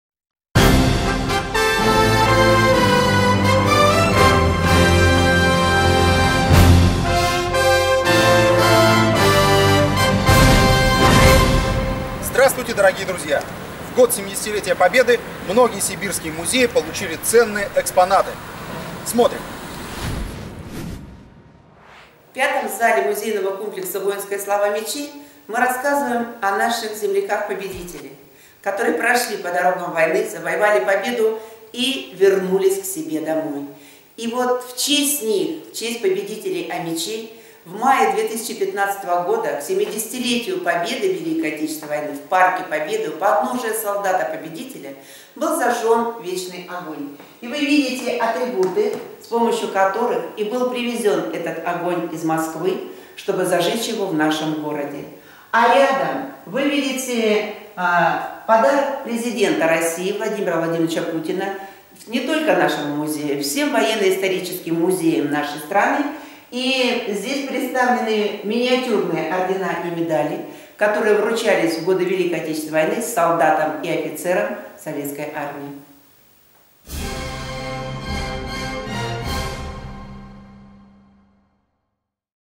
Подарок Президента России В. В. Путина. Музей Боевой славы в проекте Музеи Сибири.125
Подарок Президента России В. В. Путина. Музей Боевой славы в проекте Музеи Сибири.
В мае 2015 года в год 70-летия Победы многие сибирские музеи получили ценные экспонаты.

 В наших проектах "Музеи Сибири", "Музыка в музее", "Поэзия в музее" вы сможете увидеть много нового и интересного. Уникальные экспонаты. Талантливые музыканты, композиторы, поэты.